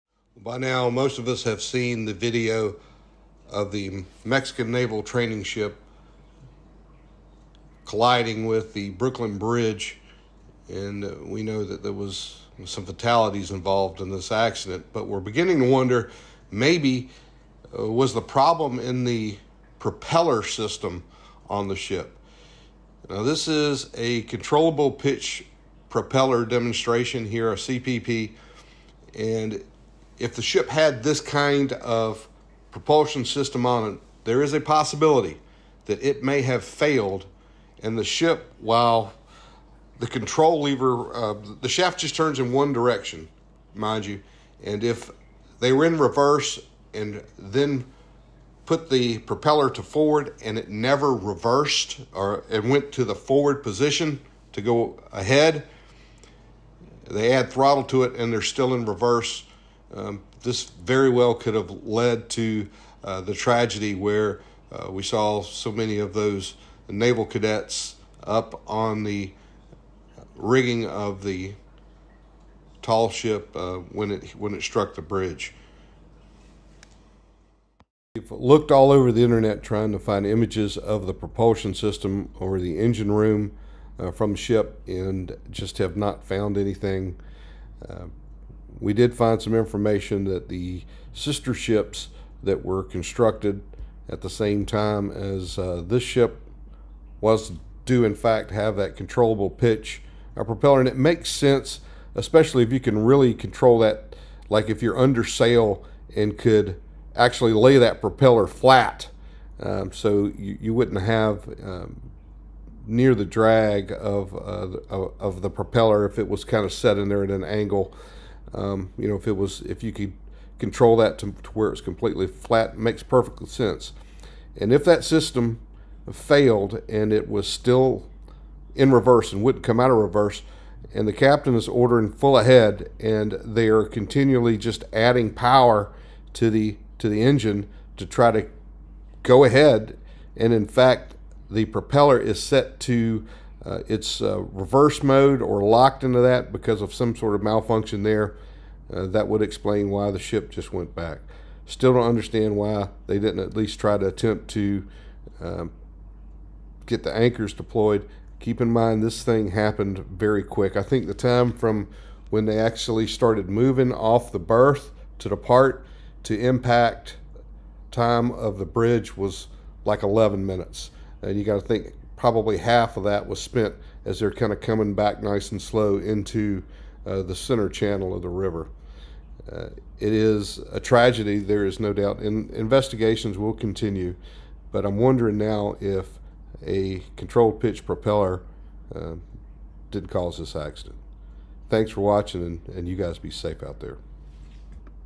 Did the CPP fail during the Mexican naval ship / Brooklyn Bridge Accident?
Another possibility as to why the ship seemed to be out of control during the time leading up to the accident.

A Controllable Pitch Propeller malfunction could be the culprit. These propulsion systems allow the propeller shaft to only need to turn in one direction, with reversing, neutral and ahead control managed by manipulating the blade faces of the propeller.

The Mexican Naval training ship Cuauhtémoc hit the underside of the Brooklyn Bridge while departing New York on May 17, 2025.

The accident will be reviewed and we will be following this event.  Video used in this report are for discussion purposes and were found on social media.